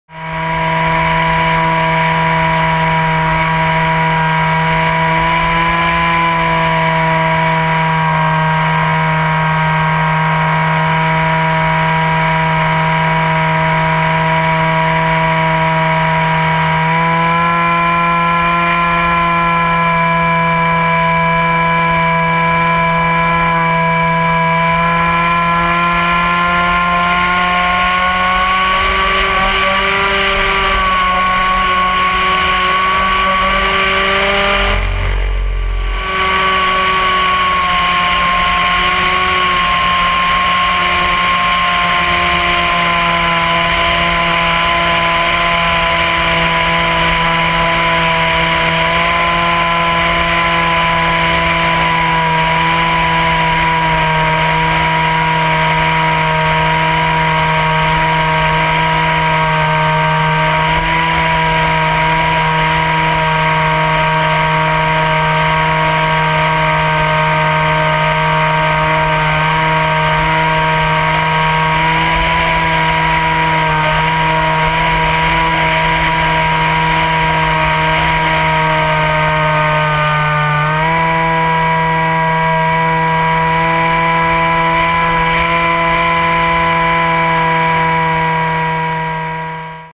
Mount Herman Proximity Run 1
My first FPV proximity run down Mount Herman near Monument Colorado.